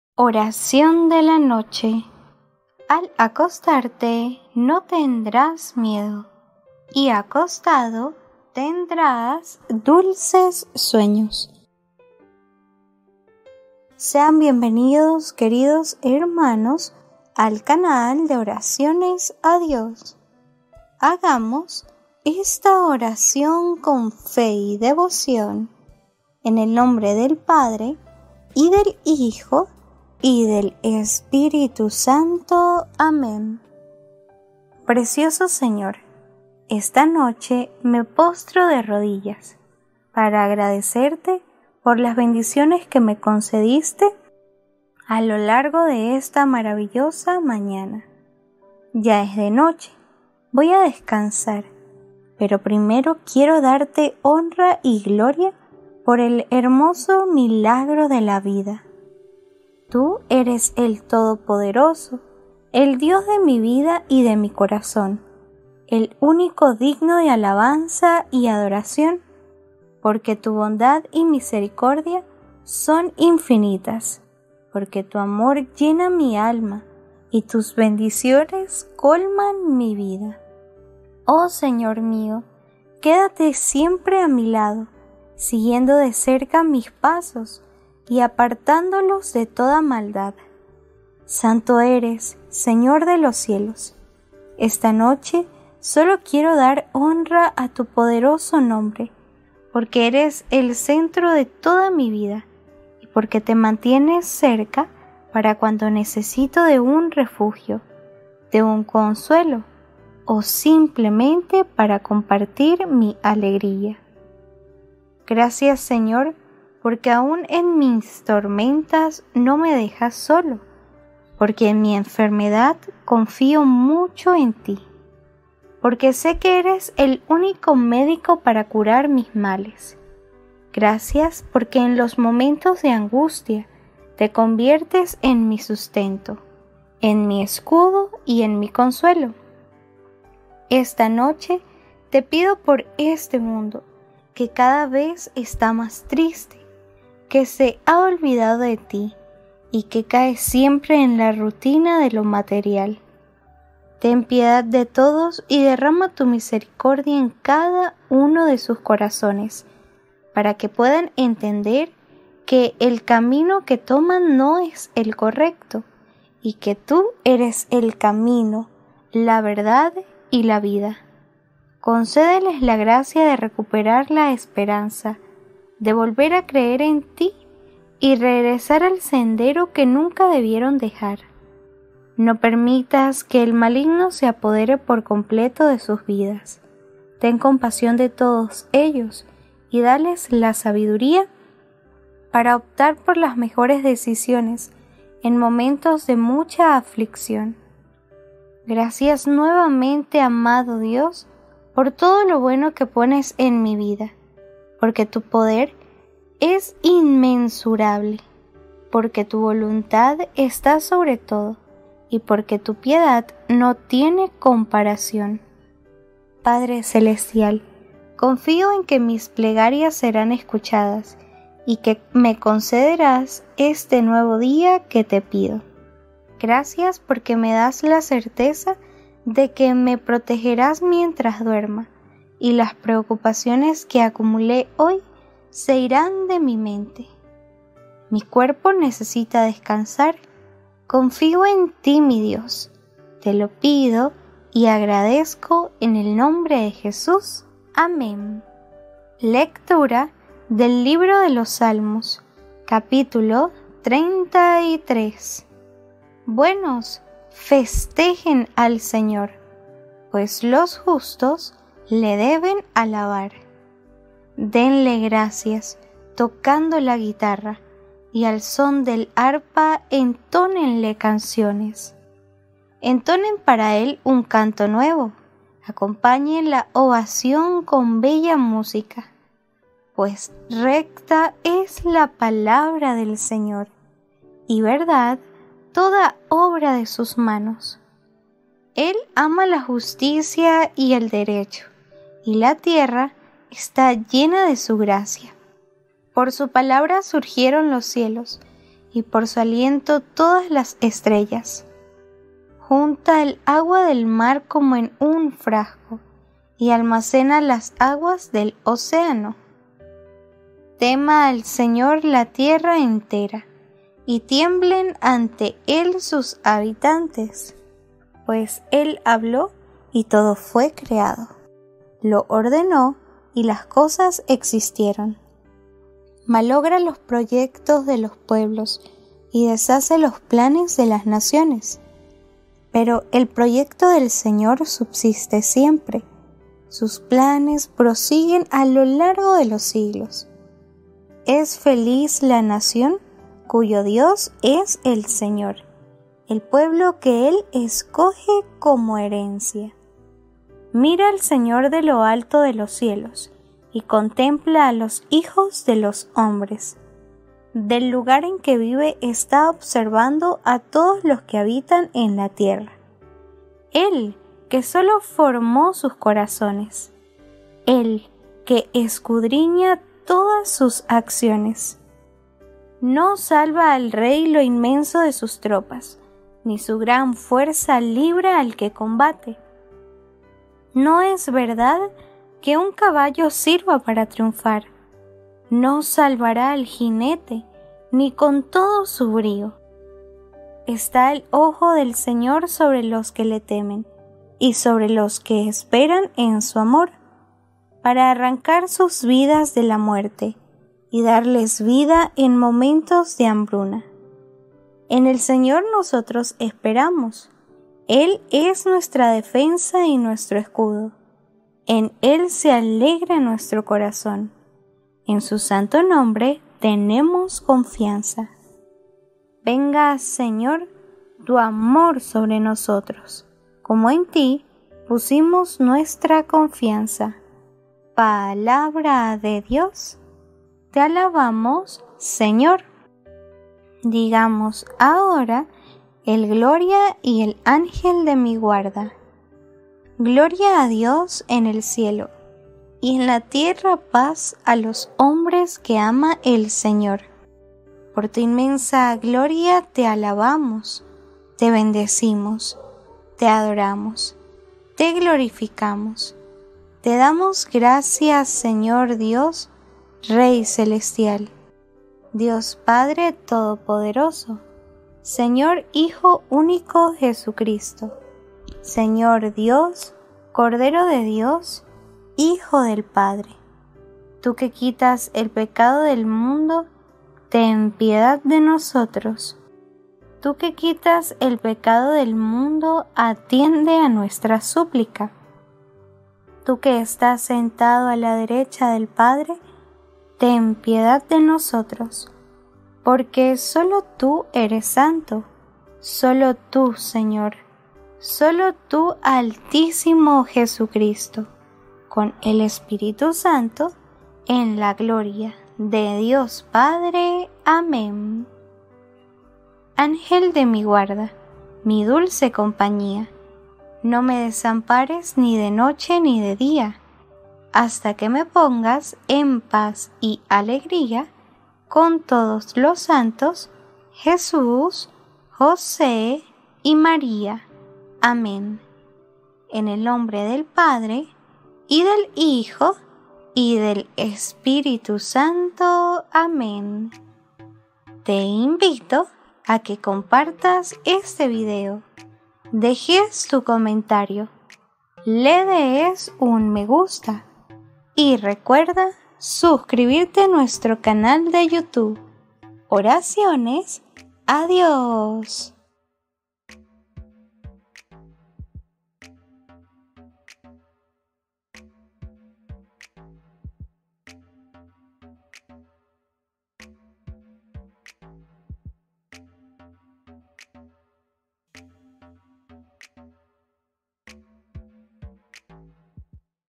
ORACION de la NOCHE de HOY 🌕 Miércoles 29 de Septiembre 2021 💤 ORACIONES A DIOS 🙏
¿quieres apoyar al padre marcos?

Al acostarte no tendrás miedo y, acostado, tendrás dulces sueños
Sean bienvenidos queridos hermanos al canal de Oraciones a Dios
Hagamos esta oración con fe y devoción

Precioso Señor, esta noche me postro de rodillas para agradecerte por las bendiciones que me concediste a lo largo de esta maravillosa mañana. Ya es de noche, voy descansar, pero primero quiero darte honra y gloria por el hermoso milagro de la vida. Tú eres el todopoderoso, el Dios de mi vida y de mi corazón. El único digno de alabanza y adoración, porque tu bondad y misericordia son infinitas, porque tu amor llena mi alma y tus bendiciones colman mi vida. Oh Señor mío, quédate siempre a mi lado siguiendo de cerca mis pasos y apartándolos de toda maldad. Santo eres, Señor de los cielos, esta noche solo quiero dar honra a tu poderoso nombre, porque eres el centro de toda mi vida y porque te mantienes cerca para cuando necesito de un refugio, de un consuelo o simplemente para compartir mi alegría. Gracias Señor, porque aún en mis tormentas no me dejas solo, porque en mi enfermedad confío mucho en Ti, porque sé que eres el único médico para curar mis males. Gracias porque en los momentos de angustia te conviertes en mi sustento, en mi escudo y en mi consuelo.
Esta noche te pido por este mundo que cada vez está más triste, que se ha olvidado de Ti y que cae siempre en la rutina de lo material. Ten piedad de todos y derrama tu misericordia en cada uno de sus corazones para que puedan entender que el camino que toman no es el correcto y que Tú eres el camino, la verdad y la vida.  Concédeles la gracia de recuperar la esperanza, de volver a creer en Ti y regresar al sendero que nunca debieron dejar. No permitas que el maligno se apodere por completo de sus vidas. Ten compasión de todos ellos y dales la sabiduría para optar por las mejores decisiones en momentos de mucha aflicción. Gracias nuevamente, amado Dios, por todo lo bueno que pones en mi vida, porque tu poder es inmensurable, porque tu voluntad está sobre todo y porque tu piedad no tiene comparación.  Padre celestial, confío en que mis plegarias serán escuchadas y que me concederás este nuevo día que te pido. Gracias porque me das la certeza de que me protegerás mientras duerma y las preocupaciones que acumulé hoy se irán de mi mente. Mi cuerpo necesita descansar, confío en Ti, mi Dios y te lo pido y agradezco en el nombre de Jesús, Amén

Lectura del libro de los salmos 33

Buenos, festejen al Señor, pues los justos le deben alabar. Denle gracias, tocando la guitarra, y al son del arpa entónenle canciones. Entonen para él un canto nuevo, acompañen la ovación con bella música. Pues recta es la palabra del Señor, y verdad toda obra de sus manos. El ama la justicia y el derecho, y la tierra está llena de su gracia por su palabra surgieron los cielos, y por su aliento todas las estrellas. Junta el agua del mar como en un frasco, y almacena las aguas del océano. Tema al Señor la tierra entera, y tiemblen ante él sus habitantes, pues él habló y todo fue creado, lo ordenó y las cosas existieron. Malogra los proyectos de los pueblos y deshace los planes de las naciones. Pero el proyecto del Señor subsiste siempre, sus planes prosiguen a lo largo de los siglos. Es feliz la nación cuyo Dios es el Señor, el pueblo que él escoge como herencia. Mira el Señor de lo alto de los cielos, y contempla a los hijos de los hombres. Del lugar en que vive está observando a todos los que habitan en la tierra; él, que solo formó sus corazones, él, que escudriña todas sus acciones. No salva al rey lo inmenso de sus tropas, ni su gran fuerza libra al que combate. No es verdad que un caballo sirva para triunfar, no salvará al jinete ni con todo su brío. Está el ojo del Señor sobre los que le temen, y sobre los que esperan en su amor, para arrancar sus vidas de la muerte y darles vida en momentos de hambruna. En el Señor nosotros esperamos, él es nuestra defensa y nuestro escudo; en él se alegra nuestro corazón, en su santo nombre tenemos confianza. Venga, Señor, tu amor sobre nosotros, como en ti pusimos nuestra confianza
Palabra de Dios, te alabamos Señor

Digamos ahora
Gloria
Ángel de mi Guarda

Te invito a que compartas este video, dejes tu comentario, le des un me gusta